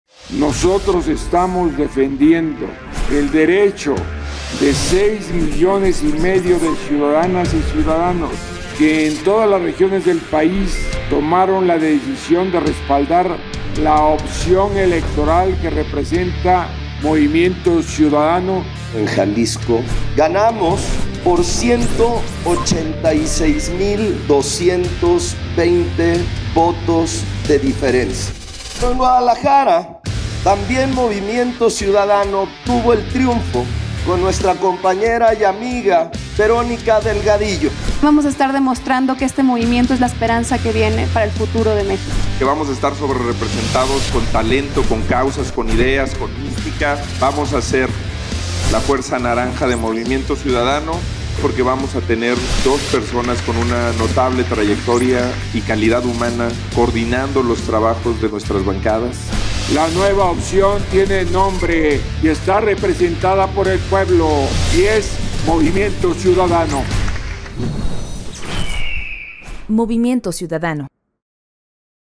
La nueva opción tiene nombre y es Movimiento Ciudadano.
En Movimiento Ciudadano estamos listos para defender las causas de más de 6.5 millones de ciudadanos que votaron por una verdadera alternativa.

No les vamos a fallar. 🍊